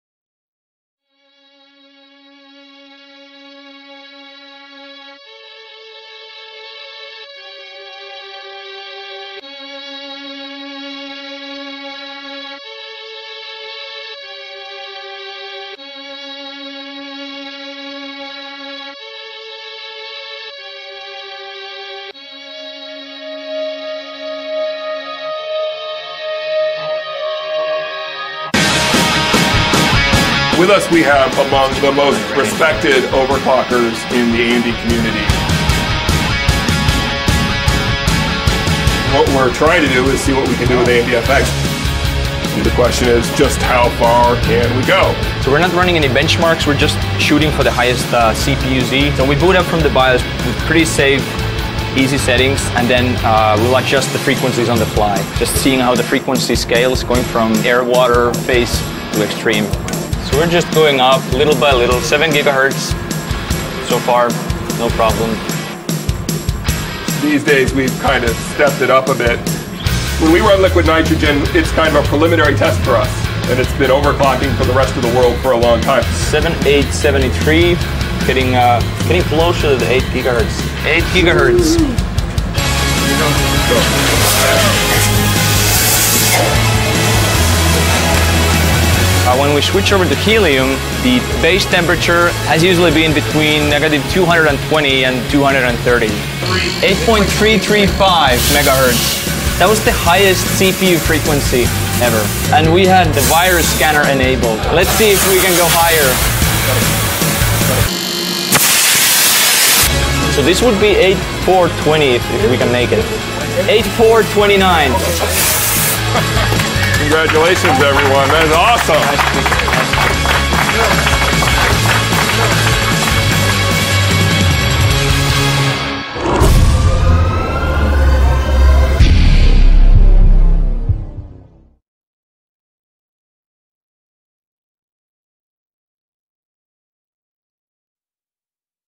Recorde De Overclock
Recorde de OC feito por um grupo especializado em OC juntamente com os proprios engenheiros da AMD sendo feito no AMD FX 8000 o recorde foi de 8.429Ghz o recorde anterior era de 8.308Ghz